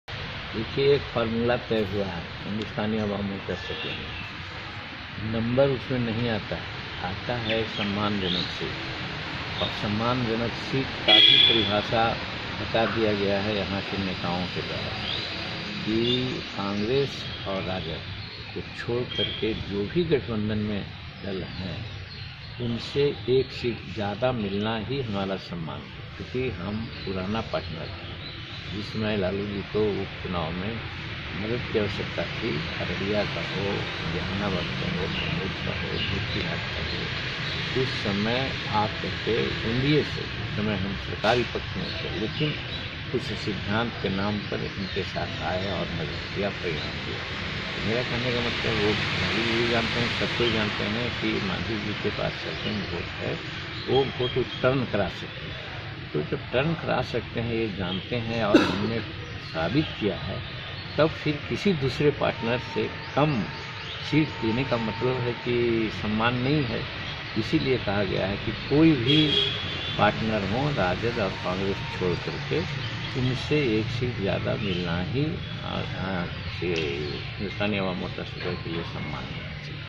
SeatSharing को लेकर JeetanramManjhi तो आर-पार के मूड में हैं, फिर दे दिया है
seatsharing को लेकर mahagathbandhan में चल रहे खींचतान के बीच jeetanram manjhi ने फिर ultimatum दे डाला है।manjhi ने साफ-साफ शब्दों में कह दिया है कि इस बार वो मानने वाले नहीं हैं क्योंकि उनका खुद का जनाधार है, क्या कहा हैं सीट को लेकर सुनिये...इस वीडियो में